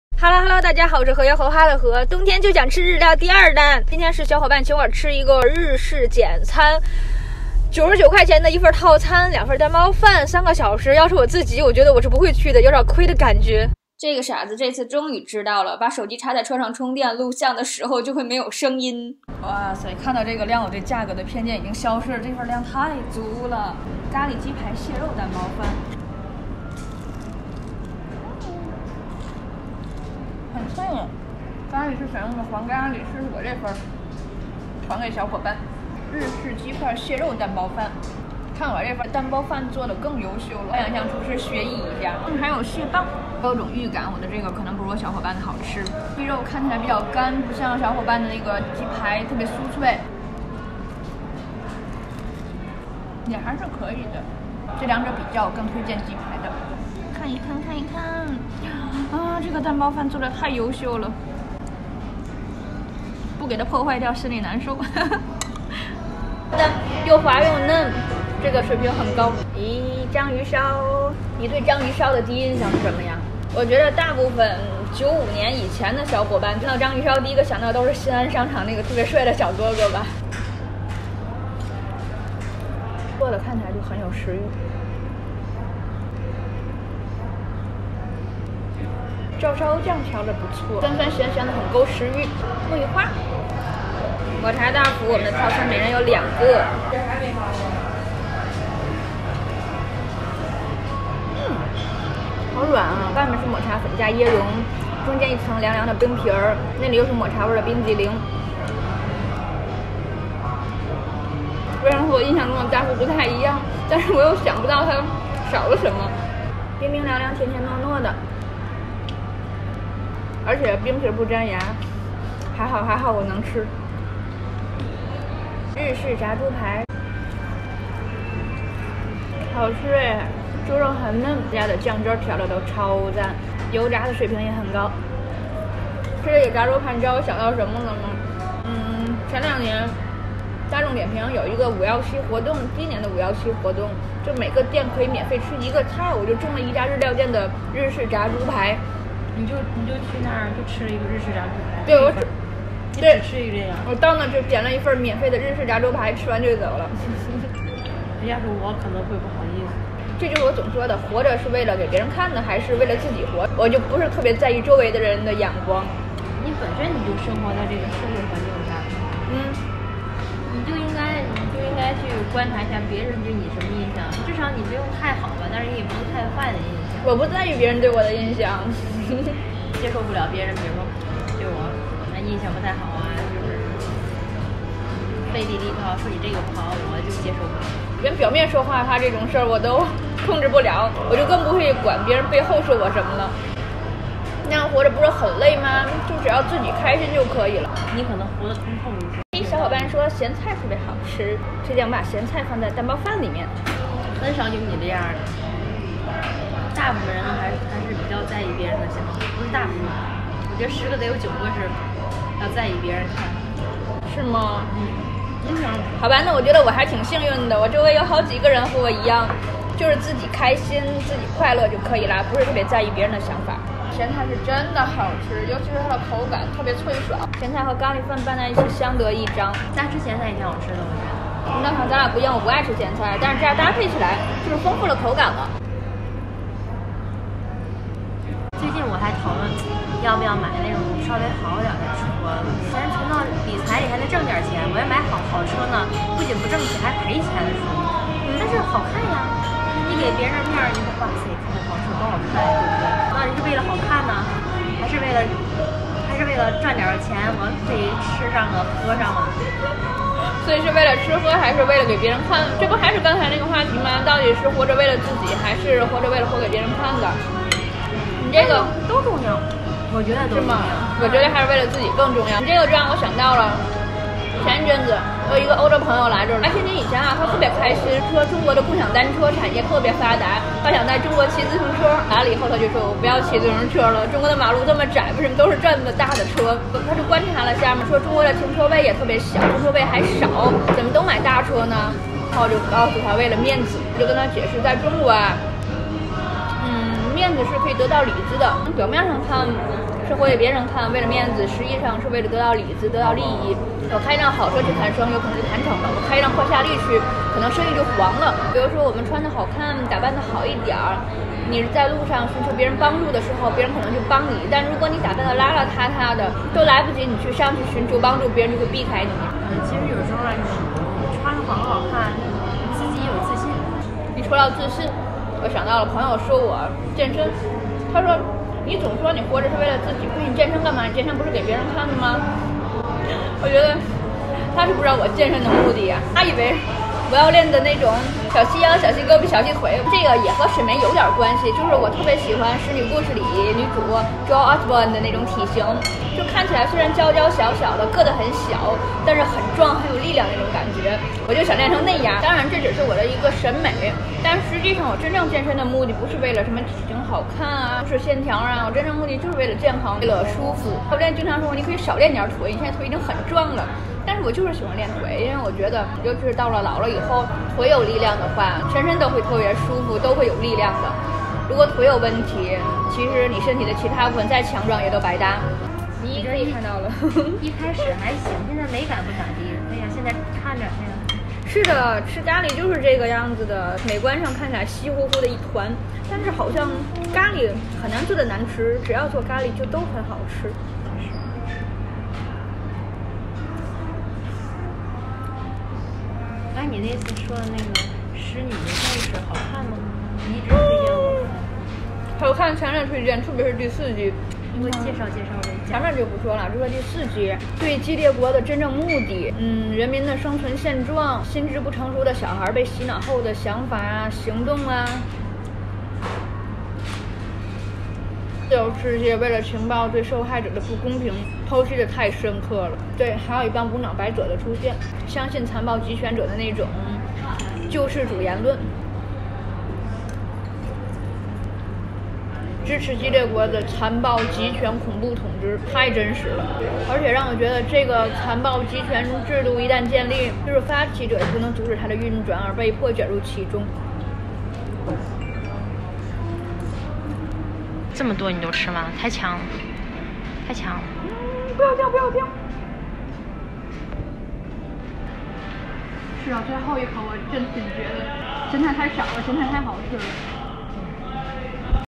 99元的双人日式简餐 血亏or超值 聊聊活着是为了自己 还是给别人看的
无欲则刚